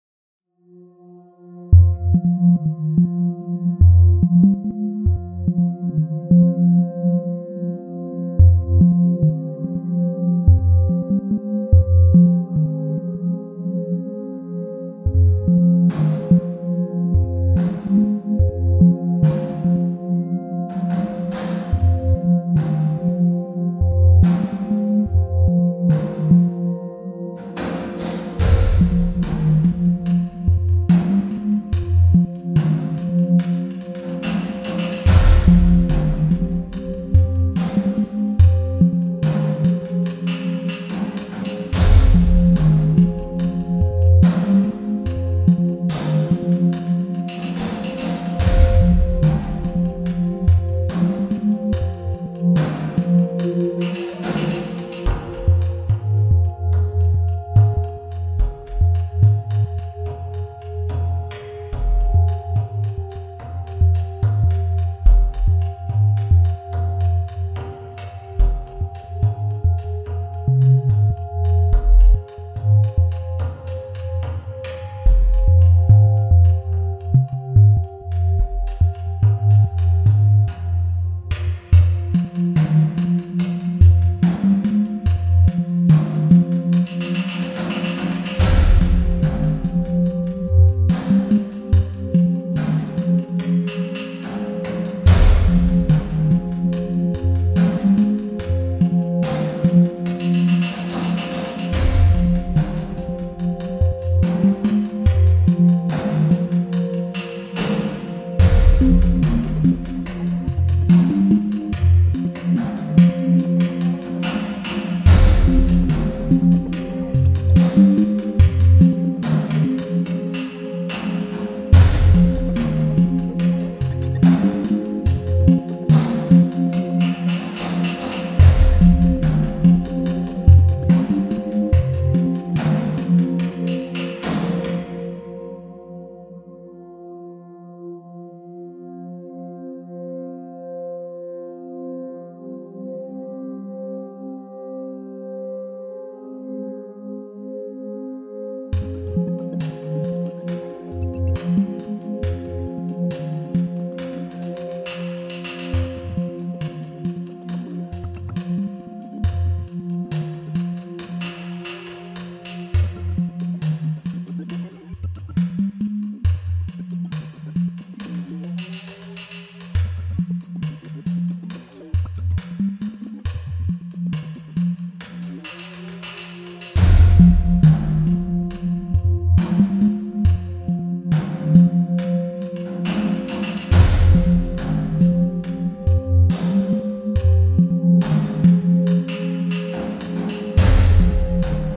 Wildlife, koalas, 'roos bears the brunt of Australian
Massive bushfires in australia a disaster for wildlife with fear the native population of some species reduced by half.